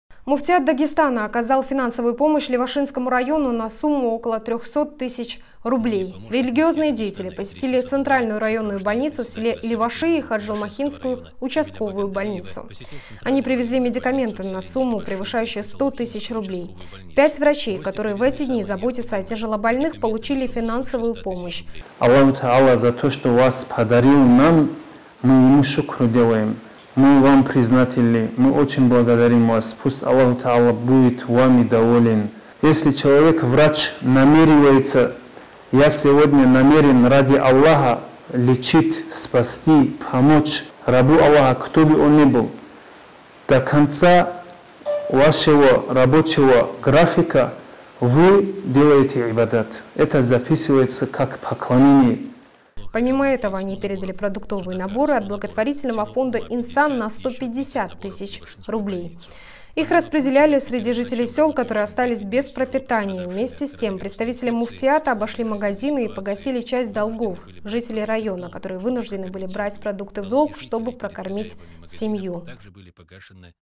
Погасили долги в магазине
Муфтият Дагестана оказал финансовую помощь Левашинскому району на сумму около 300 тысяч рублей.

Религиозные деятели посетили центральную районную больницу в селе Леваши и Хаджалмахинскую участковую больницу. Они привезли медикаменты на сумму превышающую 100 тысяч рублей. Пять врачей, которые в эти дни заботятся о тяжелобольных, получили финансовую помощь.

Помимо этого, они передали продуктовые наборы от благотворительного фонда «Инсан» на 150 тысяч рублей. Их распределили среди жителей сёл, которые остались без пропитания.

Вместе с тем представители Муфтията обошли магазины и погасили часть долгов жителей района, которые вынуждены были брать продукты в долг, чтобы прокормить семью.